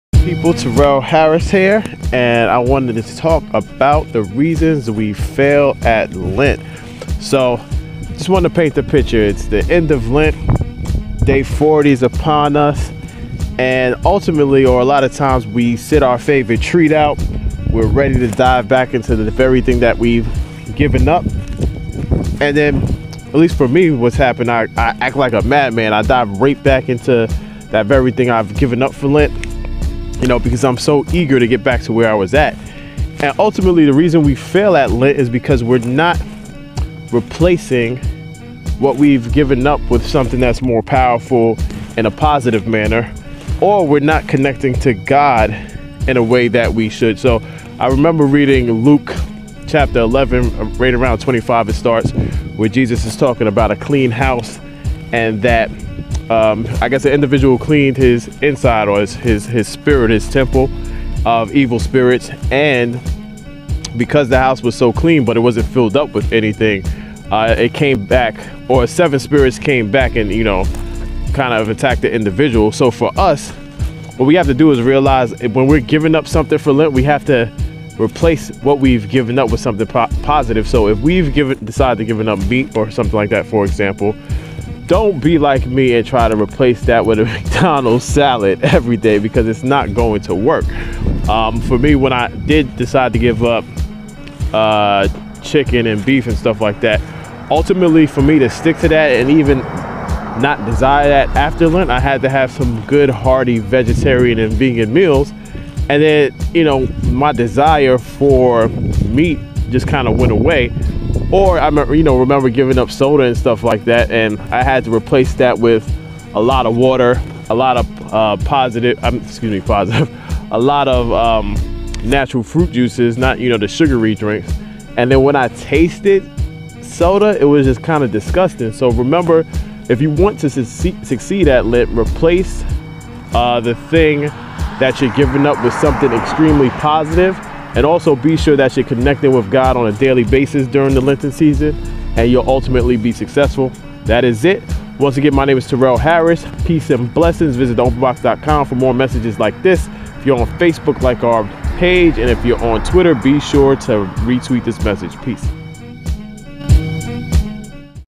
Why We fail at lent
Lent is upon us.  For Christians it is a 40 period leading up to Easter to reflect upon the crucifixion of Jesus Christ. Some of us have decided to fast, or give something up during that 40 period.  Some of us will fail at this fast.

When day 40 arrives,if we have held out this long, some of us are crazed with the excitement of diving right back into that very thing we've given up.  So why are we failing and succumbing to our urges? We fail because we don't fill ourselves up with a positive replacement for that which we have given up.

For instance when I gave up meat for one fast, I was like a savage animal eating McDonald's salads until someone introduced me to hardy vegan and vegetarian meals.  My animalistic behavior would soon subside. There's a spiritual dynamic to this too.  Starting at Luke 11:25, Jesus speaks of a man having a empty, clean, spiritual house.  Because the man didn't fill it up with the spirit of God, 7 unclean spirits moved right in, making the man worse than before.

If you want to succeed during lent always add something positive during that 40 period.